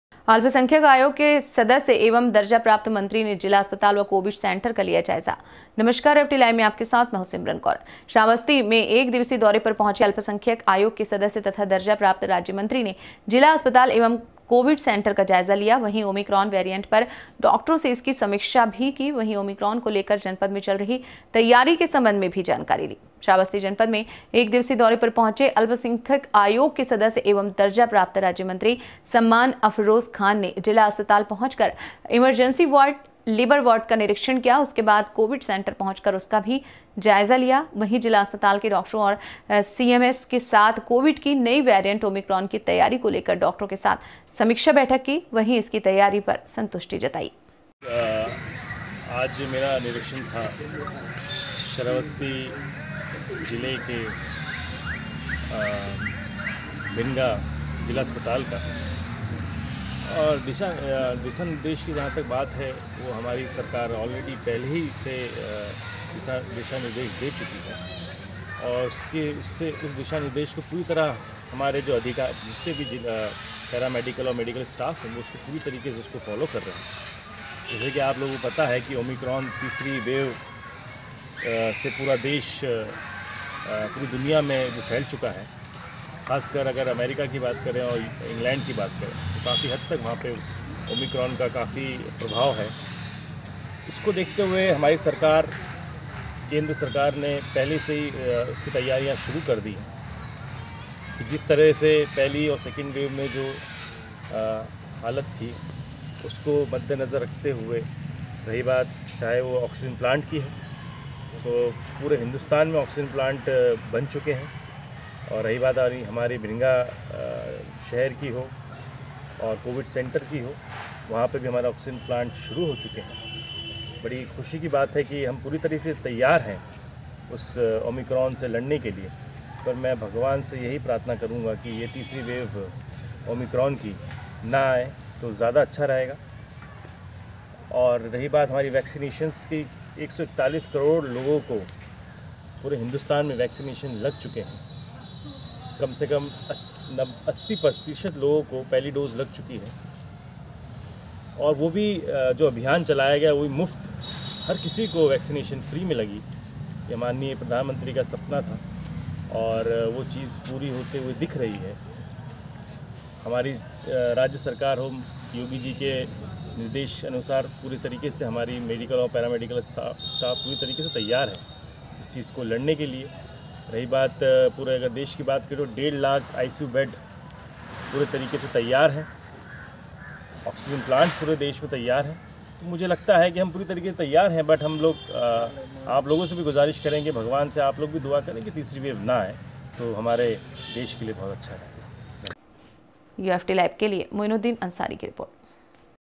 अल्पसंख्यक आयोग के सदस्य एवं दर्जा प्राप्त मंत्री ने जिला अस्पताल व कोविड सेंटर का लिया जायजा।
Story idea

Place:-Shravasti

Report: मुईनुद्दीन अंसारी श्रावस्ती

Slug:-अल्पसंख्यक आयोग के सदस्य एवं दर्जा प्राप्त मंत्री ने जिला अस्पताल व कोविड सेंटर का लिया जायजा।

Anchor:-श्रावस्ती एक दिवसीय दौरे पर पहुंचे अल्पसंख्यक आयोग के सदस्य तथा दर्जा प्राप्त राज्यमंत्री ने जिला अस्पताल एवं कोविड सेंटर का जायजा लिया वहीं ओमीक्रान वेरियंट पर डॉक्टरों से इसकी समीक्षा भी की वही ओमीक्रान को लेकर जनपद में चल रही तैयारी के संबंध में भी जानकारी ली

V/O:-श्रावस्ती जनपद में एक दिवसीय दौरे पर पहुंचे अल्पसंख्यक आयोग के सदस्य एवं दर्जा प्राप्त राज्यमंत्री सम्मान अफरोज खान ने जिला अस्पताल पहुंचकर इमरजेंसी वार्ड, लेबर वार्ड का निरीक्षण किया उसके बाद कोविड सेंटर पहुंचकर उसका भी ज्याजा लिया वहीं जिला अस्पताल के डॉक्टरों और सीएमएस के साथ कोविड की नये वेरिएंट ओमीक्रान की तैयारी को लेकर डॉक्टरों के साथ समीक्षा बैठक की वंही इसकी तैयारी पर संतुष्टि जताई।

बाइट:-सम्मान अफ़रोज़ खान(अल्पसंख्यक आयोग के सदस्य एवं राज्यमंत्री)